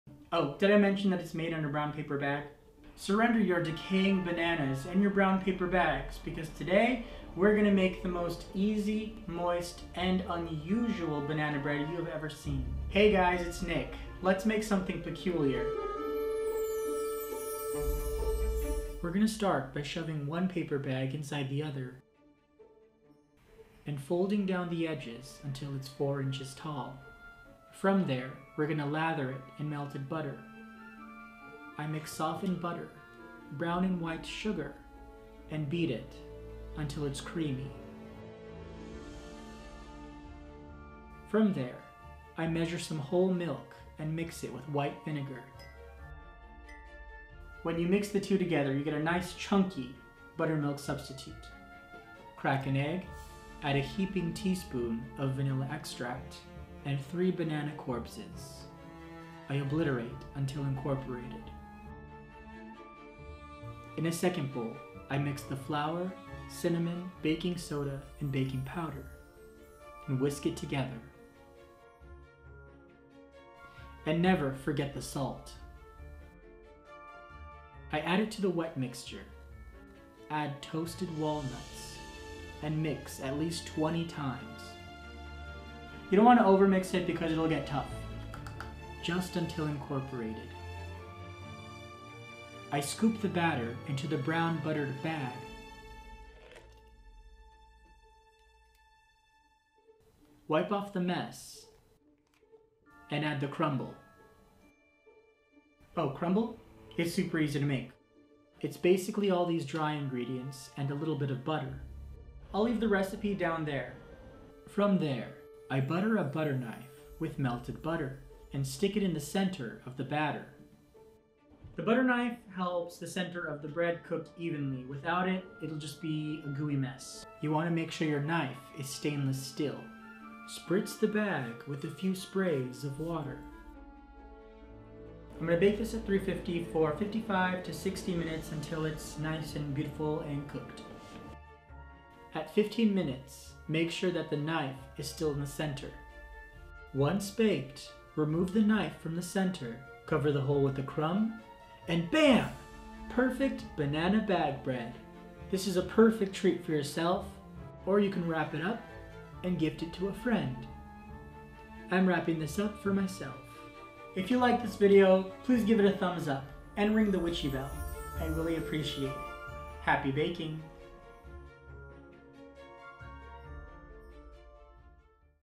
Banana Bag Bread! | Weird baking method
Banana bread is a classic favorite. This banana bread is moist, easy to make, and baked in a BROWN PAPER BAG! Why make it in a brown paper bag? Well, this particular method is great for those who like to gift and those who don't care to buy a loaf pan. It is a simple, easy, and eye catching method to wow your friends and family. Plus it just looks to cool!!! My question to you is, is it bread or a muffin?

INGREDIENTS
1/2 cup Butter (softened)
1/4 cup White Sugar
1/2 cup Brown Sugar
3 very ripe Bananas
1 tsp Vanilla (heaping)
1 Egg
1/2 cup Whole milk
1Tbsp White Vinegar
1 1/2 cups Flour (all purpose)
1 tsp Baking soda
1 tsp Baking powder
1 tsp Cinnamon
1/4 tsp Salt
1/2 cup toasted Walnuts (optional)

Other supplies
Brown paper bags (acid free and food safe)
Butter knife (stainless steel)
Pastry brush
Cellophane wrap (for gifting)

Crumb Topping
3 Tbsp Flour (all purpose)
1 1/2 Tbsp Brown Sugar
1/8 tsp Salt
1 1/2 Tbsp Butter (melted)
1/2 tsp Cinnamon
1/4 cup Crushed Walnuts or Oats

For crumble topping,
Mix all ingredients together until chunks form. Freeze for 10 minutes before adding on top of the banana bread.

Bake at 350°f 55-60 minutes,

Notes*
*Let cool completely before removing the butter knife from the center and cutting. About 2 hours. The bread sets during cooling time.

*Make sure your butter knife is stainless steel.

* Don't over wet the paper bag before baking. Just a few spritzes.

*Check on the bread once at 15 minutes to adjust the knife in the center and again at 30 minutes to make sure everything is looking good.

* Roll bag to be 4 inches

Tag me on social! I'd love to see your creations!

Magic is in the simplest of ingredients.

Peculiarly Yours,
Nikk